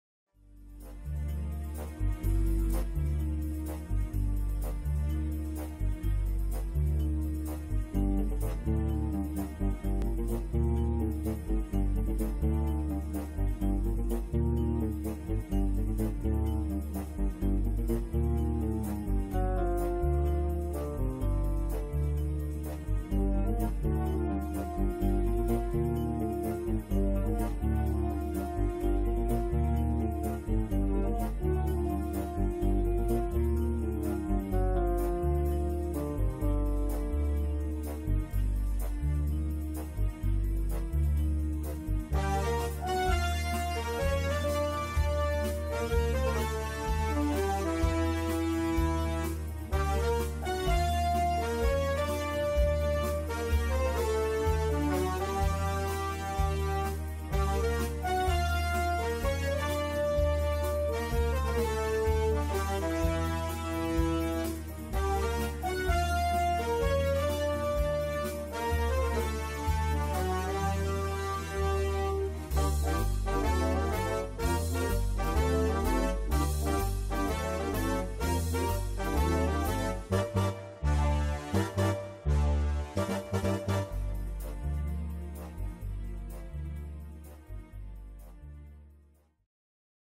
Bond, James Bond 1 - Rectangular Art by John Robert Beck
This art was created in 2013. This piece is from the first Bond film "Doc No" starring Sean Connery. This is the scene where Bond says the classics words "Bond, James Bond".

In this video, you will see the rectangular version and an recreation of how it was made.